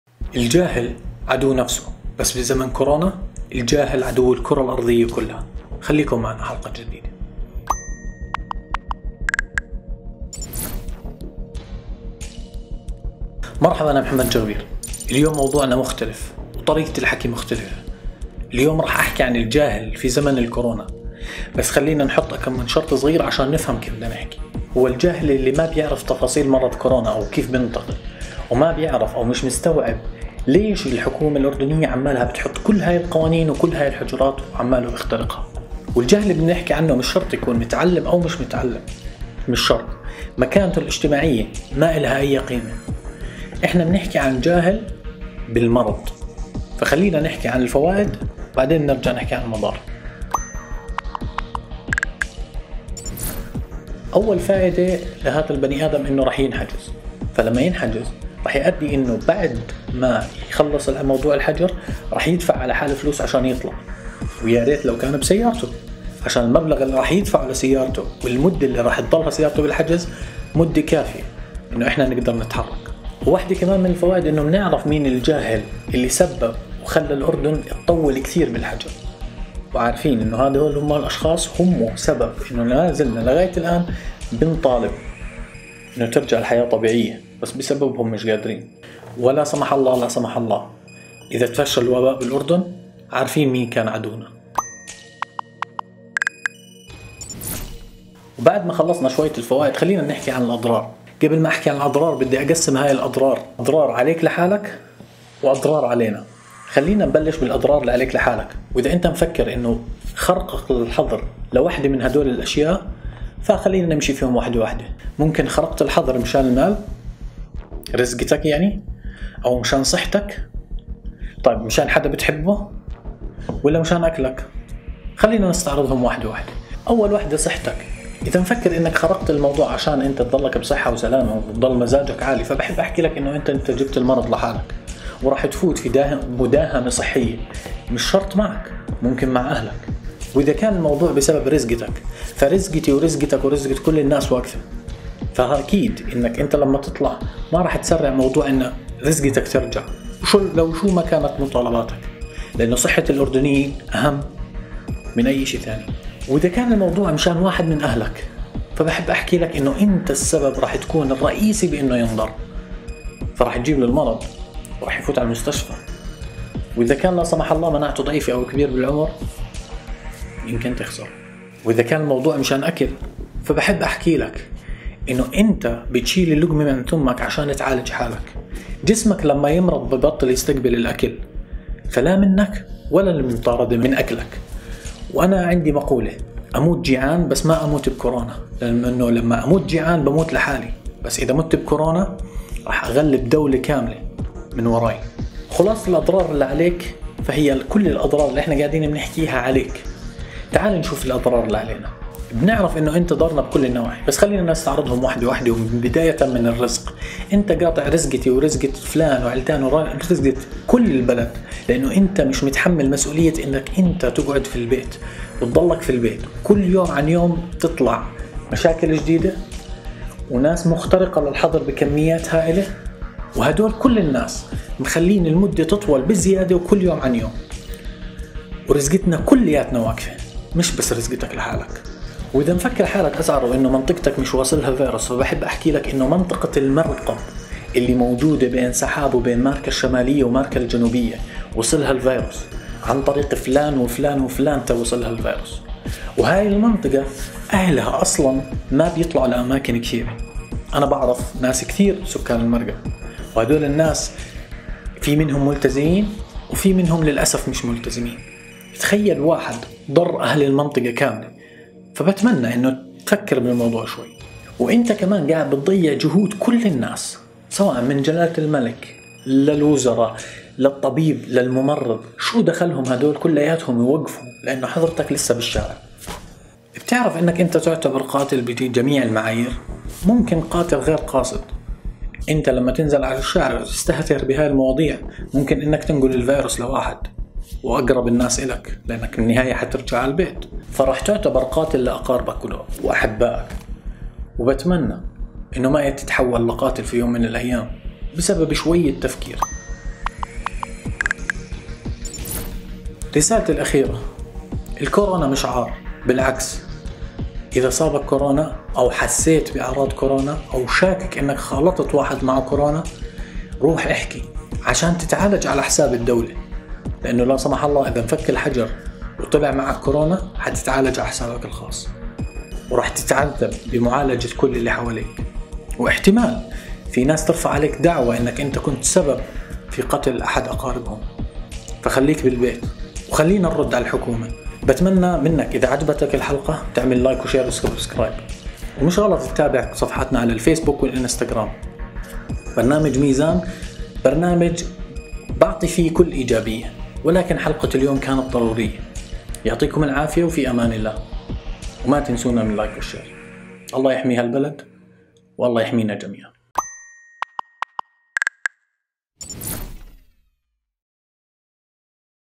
ميزان - الحلقة 4 - الجهل في زمن الكرونا
الجهل في زمن الكرونا
حلقة مميزة تختلف بطبيعتها عن الباقين
بتمنى تشير الفيديو سواء اتفقت معي او اختلفت معي وتكتب وجهه نظرك